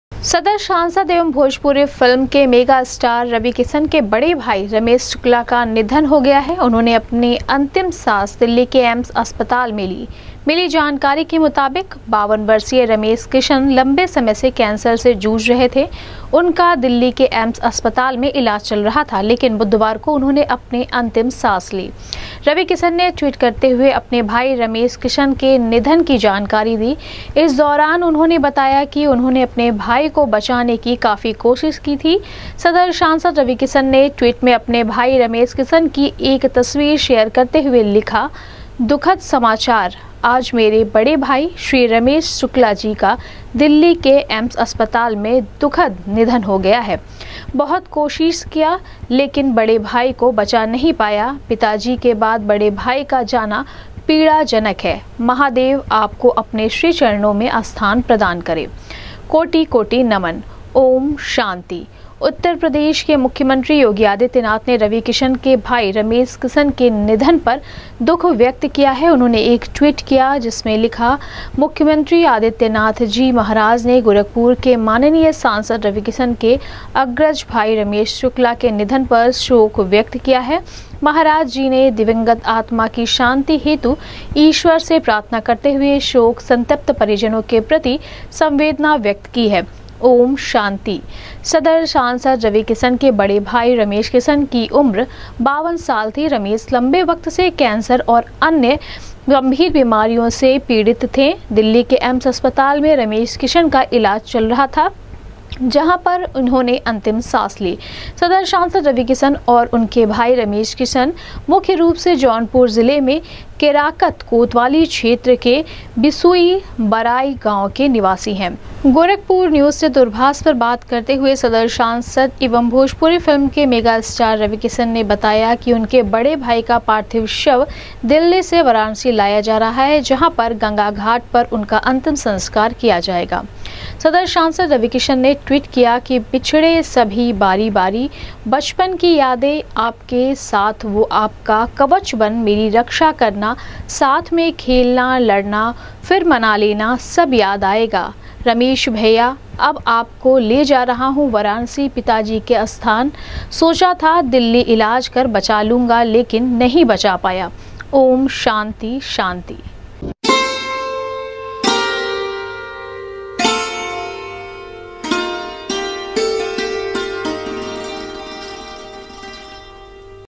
सदर सांसद एवं भोजपुरी फिल्म के मेगा स्टार रवि किशन के बड़े भाई रमेश शुक्ला का हुआ असामयिक
सदर सांसद एवं भोजपुरी फिल्म के मेगा स्टार रवि किशन के बड़े भाई रमेश शुक्ला का हुआ असामयिक निधन, मुख्यमंत्री योगी आदित्यनाथ ने दिवंगत आत्मा की शांति हेतु ईश्वर से प्रार्थना करते हुए शोक संतप्त परिजनों के प्रति व्यक्त की गहरी संवेदना ।।